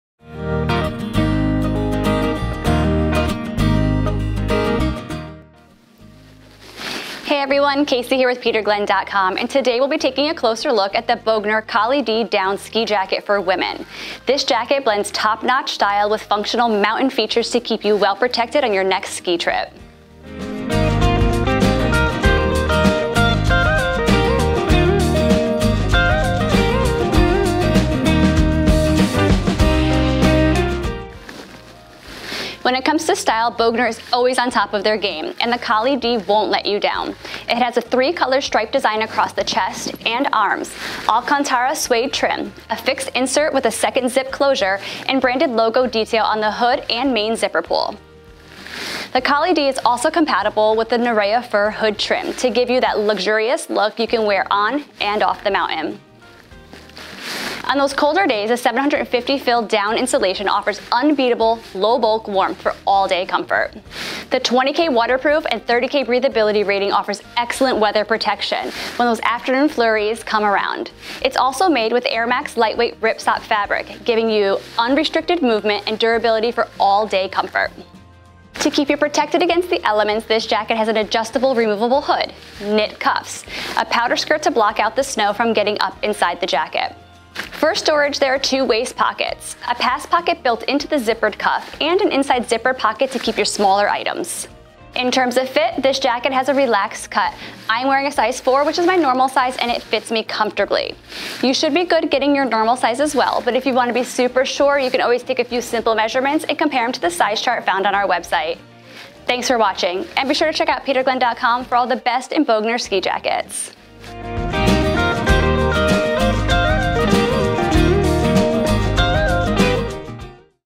Bogner Colly-D Down Ski Jacket Review
The Bogner Colly-D Down Ski Jacket blends top-notch style with functional mountain features.